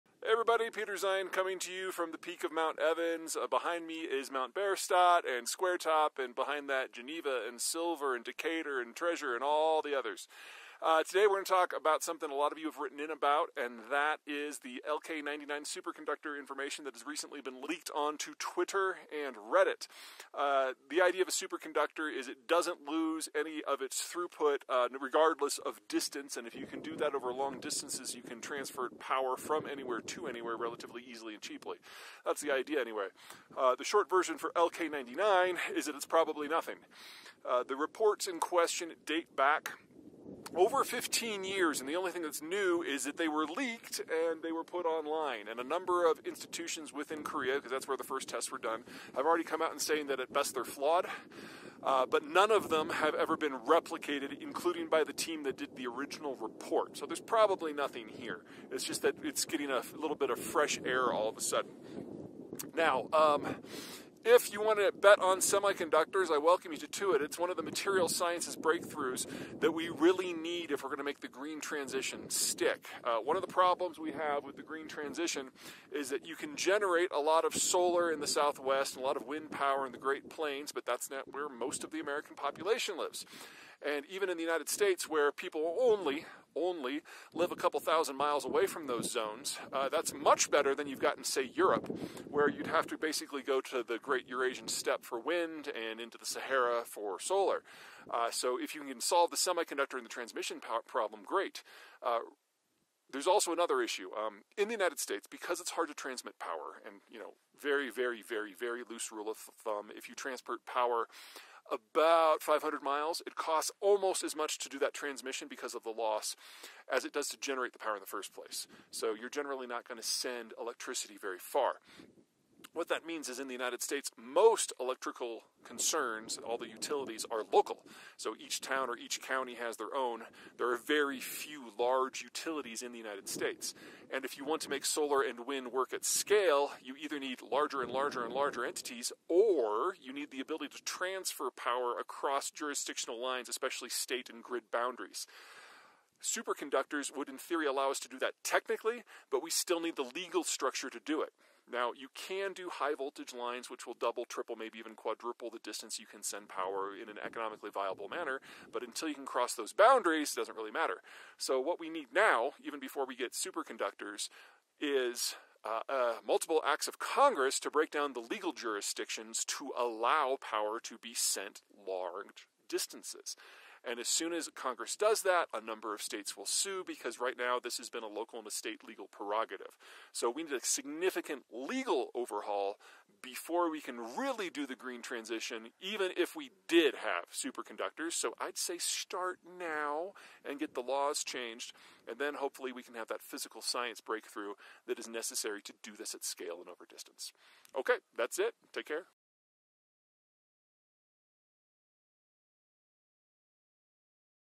Is the LK-99 Superconductor the Key to Green Energy? || Peter Zeihan
Is the superconductor of every Green's dreams finally here? I hate to burst your bubble, but the LK-99 is just too good to be true.